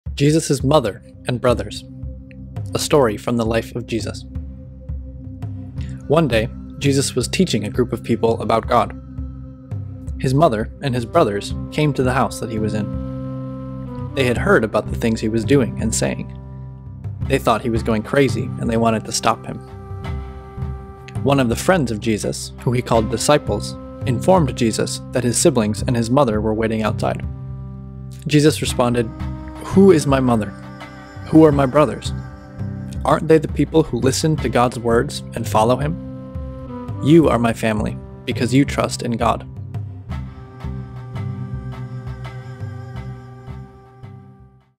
Jesus’ Mother and Brothers
Bible Story for Children

Luke 8:19-20
19 His mother and brothers came to him, and they could not come near him for the crowd. 20 Some people told him, “Your mother and your brothers stand outside, desiring to see you.” 21 But he answered them, “My mother and my brothers are these who hear the word of God, and do it.”